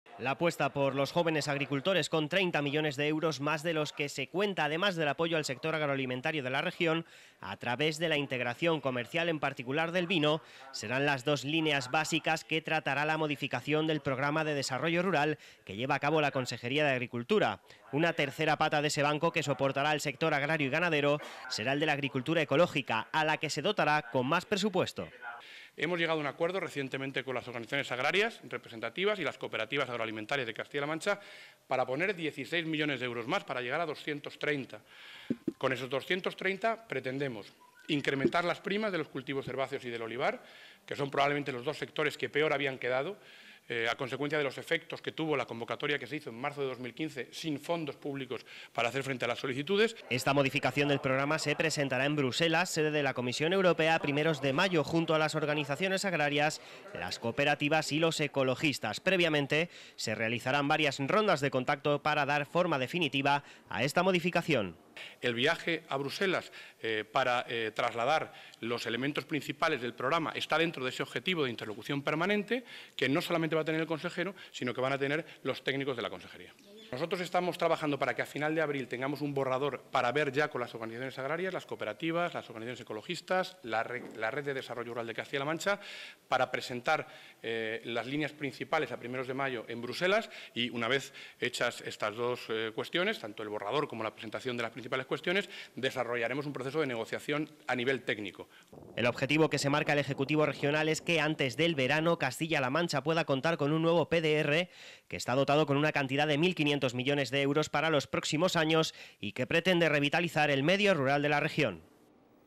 BRUSELAS DEBE DAR EL VISTO BUENO AL NUEVO PROGRAMA DE DESARROLLO RURAL
16 millones de euros más para agricultura ecológica. Es uno de los principales cambios que el gobierno de Castilla-La Mancha quiere introducir en su programa de desarrollo rural. A primeros de mayo la Consejería presentará en Bruselas esta modificación para que cuente con su visto bueno. el programa podría ver la luz antes del verano.